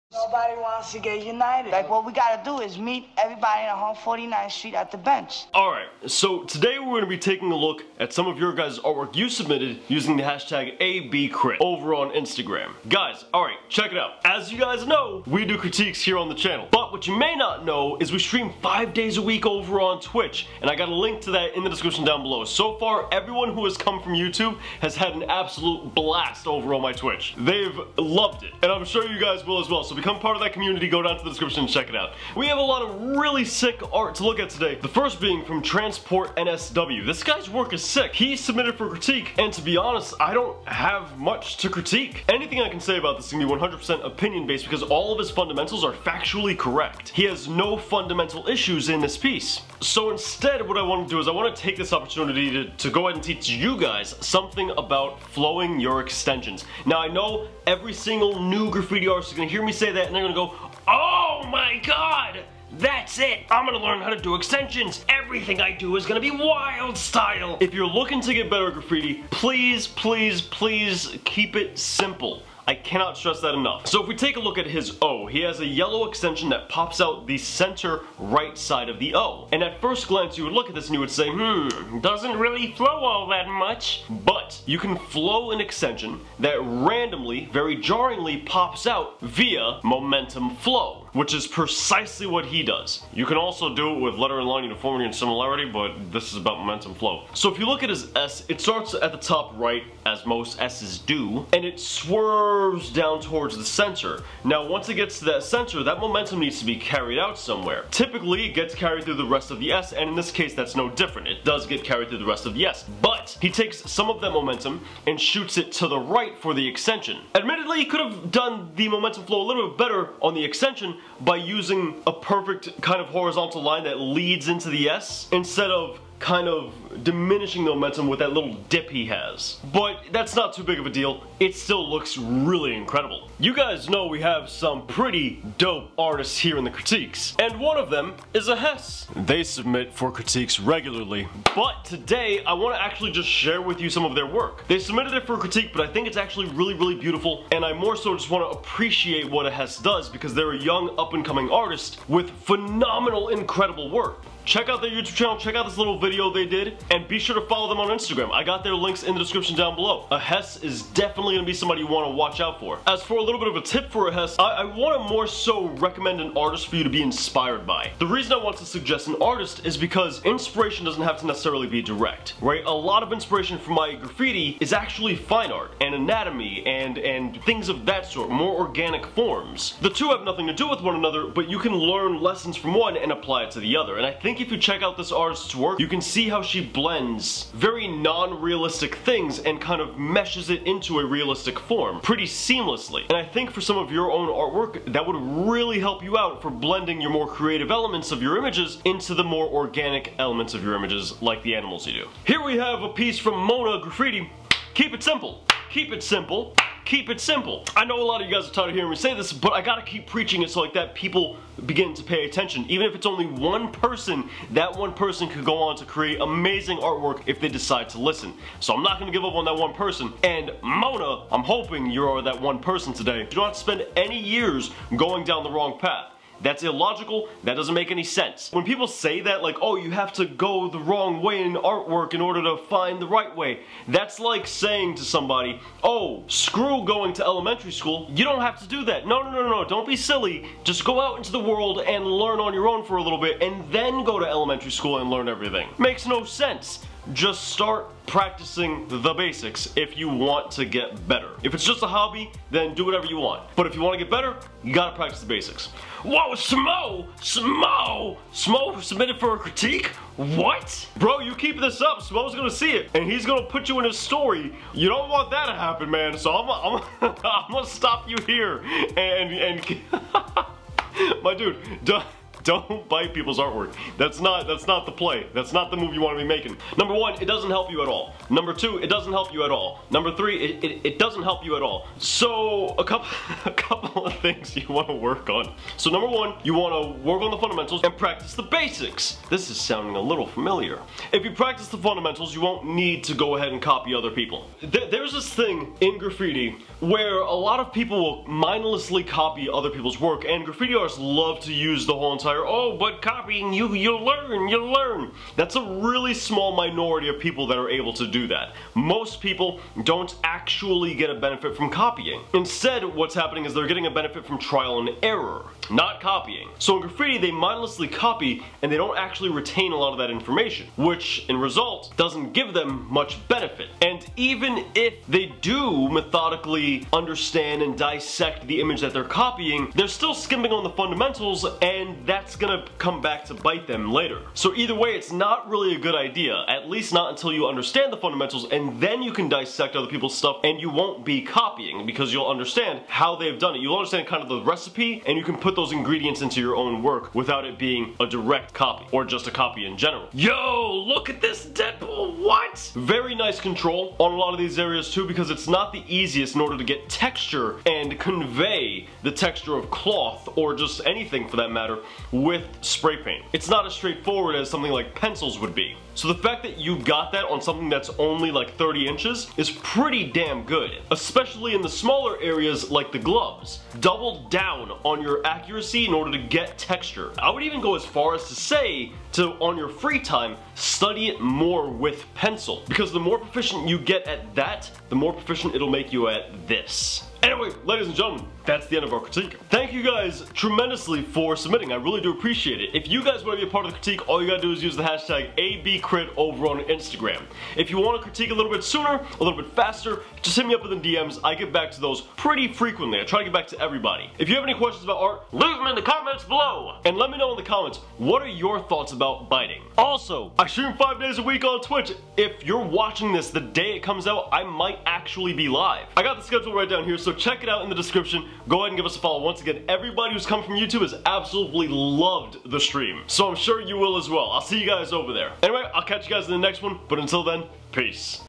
Calling Out Biting Graffiti Artist! How To Do Extensions!
Artists

In today's art/graffiti video critique we take a look at some more of your guy's amazing artwork. I unfortunately had to call out a graffiti artist here for biting someones work in hopes they go on to create their own original amazing graffiti. The artists in today's video are unbelievably good, with not only spray paint but with brush and markers too. In today's graffiti critique I even teach you guys how to do extensions in graffiti.